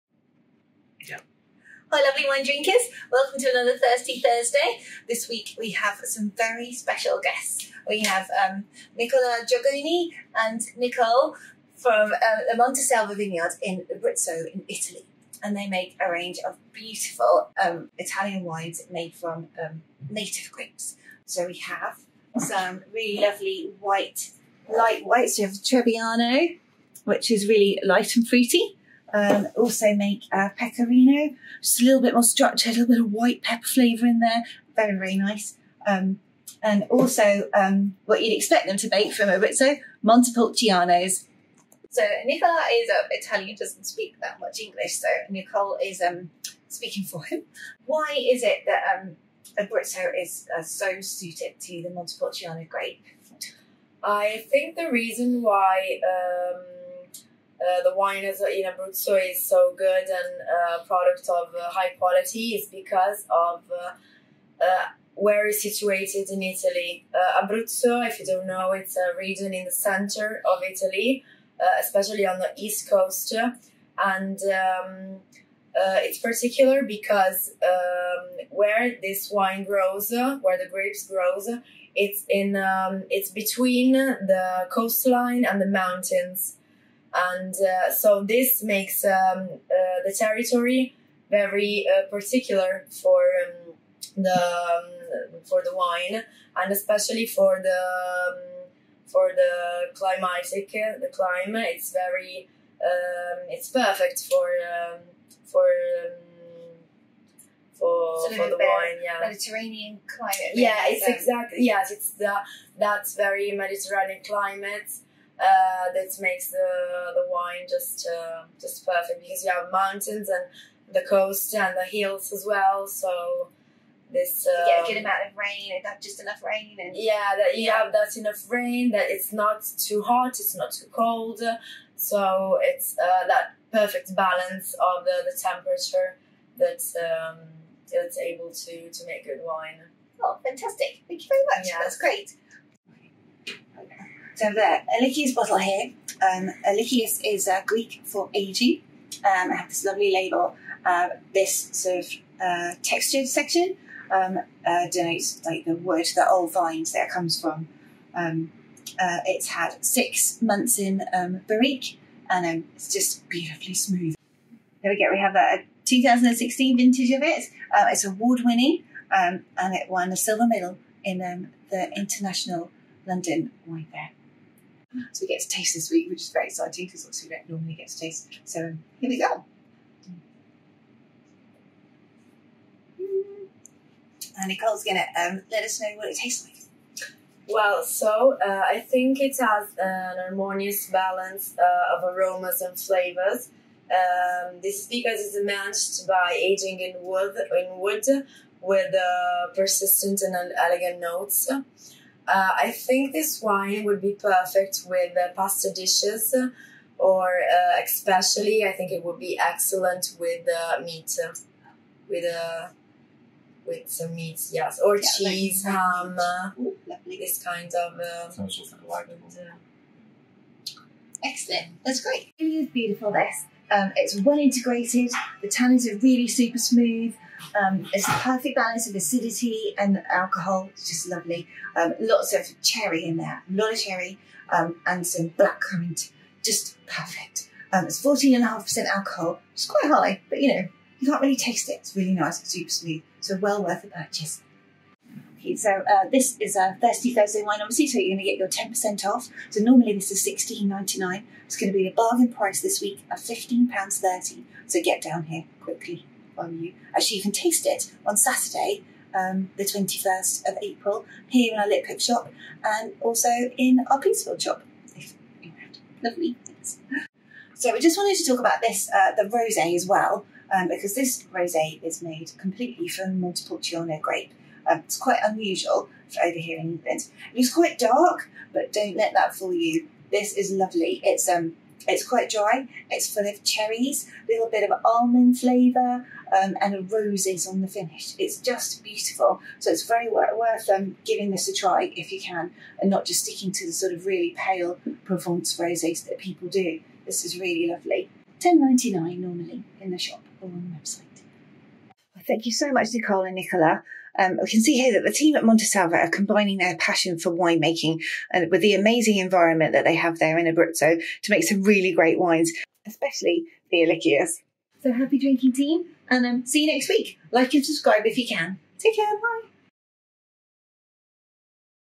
The General Wine Company, Wine recommendation, Monteselva 'Ilikias' Montepulciano d'Abruzzo
Wine Recommendation and Food Pairing, Monteselva 'Ilikias' Montepulciano d'Abruzzo, Interview with Monteselva and ACTUAL tasting this week, yippee!

Tasting Notes:  Wine obtained from a blend of Montepulciano d'Abruzzo grapes from old vines, coming from hilly areas of the Abruzzo coast, with a harmonious balance of aromas and flavours, enhanced by ageing in wood with persistent and elegant notes.  Brimming with cherry fruits with a touch of spice, perfectly balanced and super smooth.

Why is Abruzzo so perfect for growing Montepulciano grapes?  Find out here!